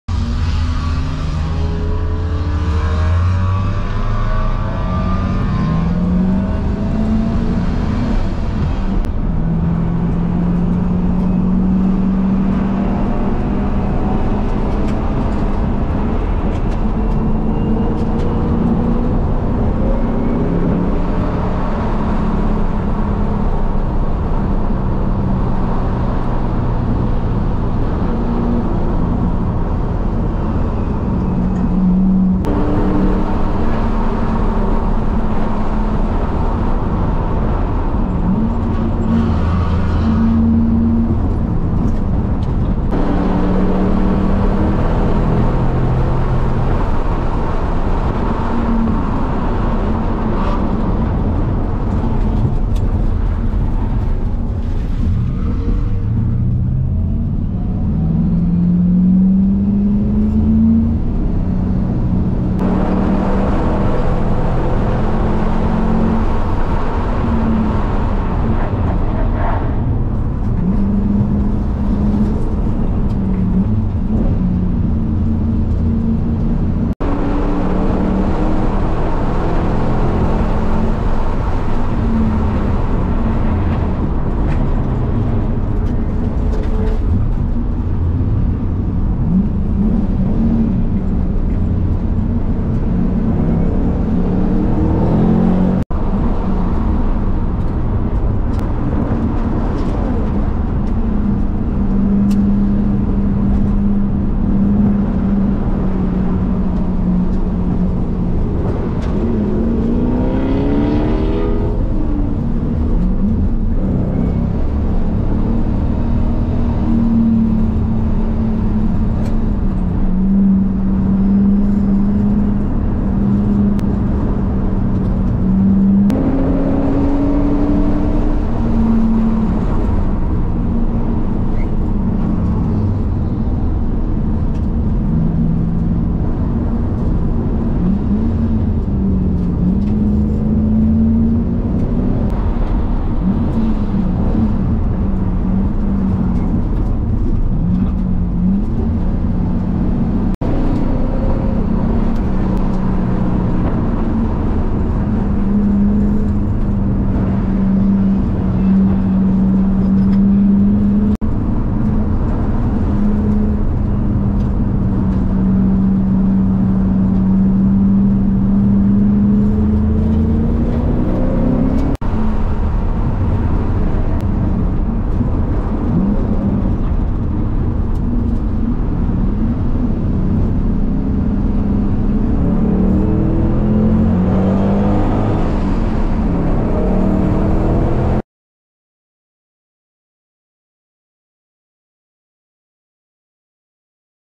Rockingham HPDE 2 Woopsies
October 15, 2022.  I've run off of tracks a lot of times and I wish somebody had filmed me doing it because I'd love to see it.  So...